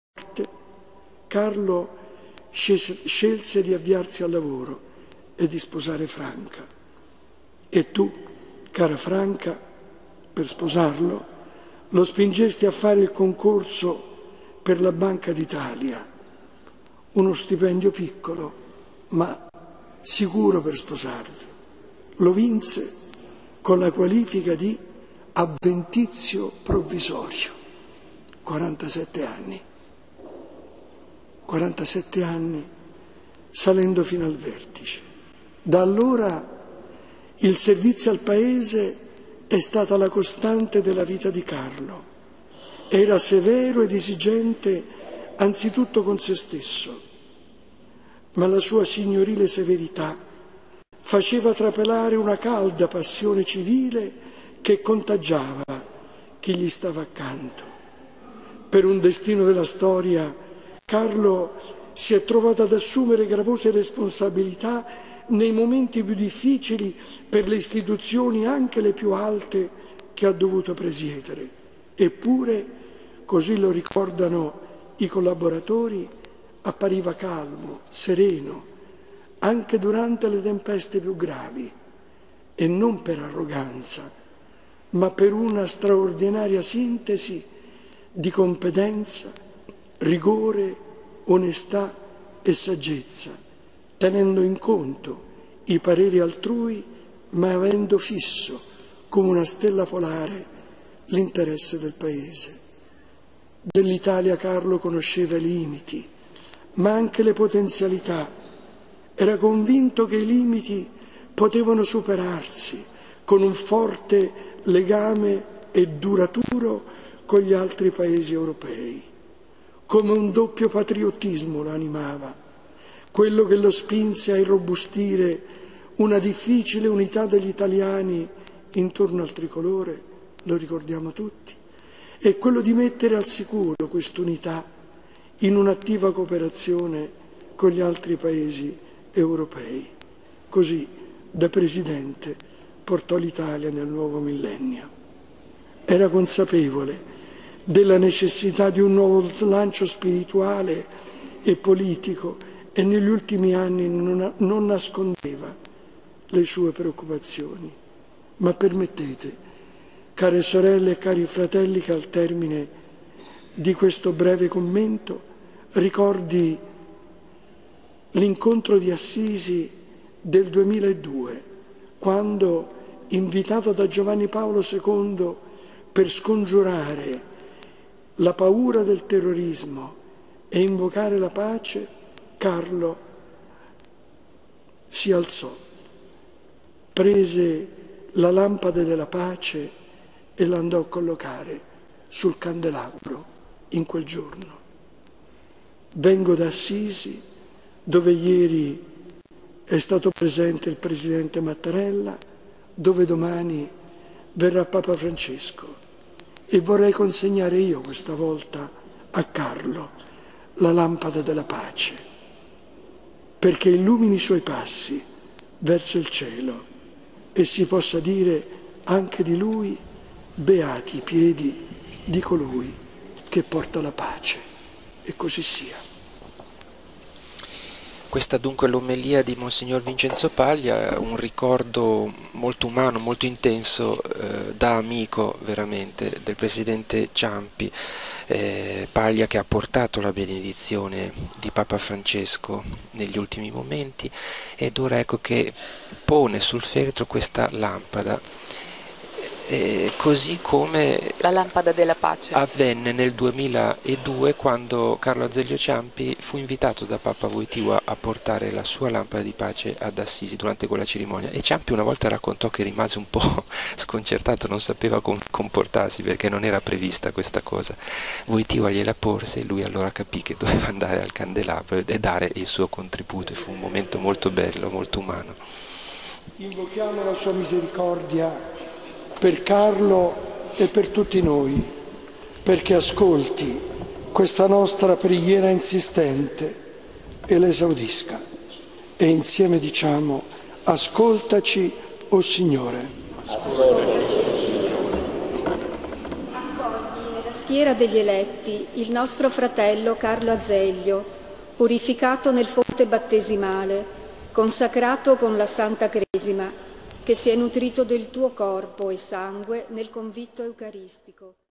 Funerale di Carlo Azeglio Ciampi
Roma, chiesa di San Saturnino martire
19 settembre 2016

L'omelia dell'arcivescovo Vincenzo Paglia